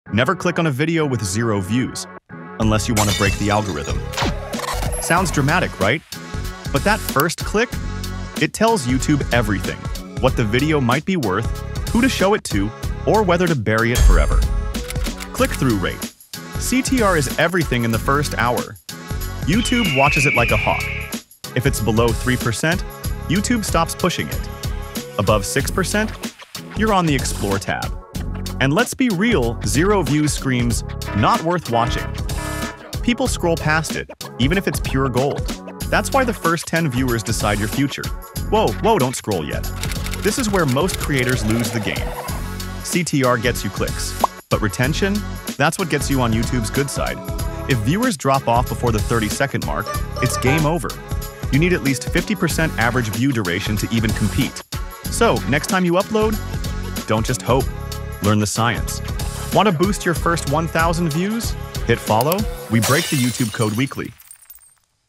What Happens When You Click on a YouTube Video with 0 Views?
Ever wondered what happens when you click on a YouTube video with 0 views?

Watch this video to find out how the YouTube algorithm works and how you can help break the 0 view mark.

 Learn how CTR and audience engagement play a role in getting your video to 1000 views!